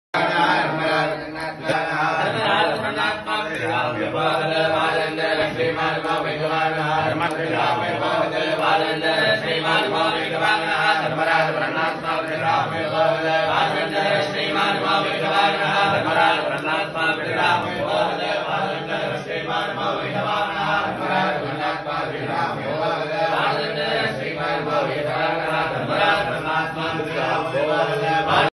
Yantra Pooja I யந்திரம் வழிபடும் I Nadi Astrology Remedies - Tirikala.com
Karma rectification solution identified during Nadi astrology Palm Leaf Reading  and pooja has been performed with God grace before Shipment  for Yantra..